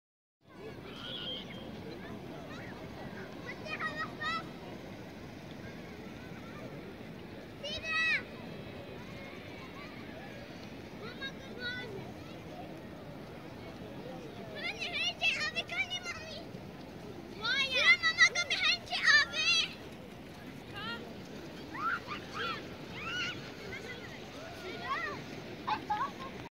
Oostende (Ostend) Beach in Belgium
Its location is in Flanders part of Belgium. In this video, you are seeing North Sea. This beach is really nice especially during summer.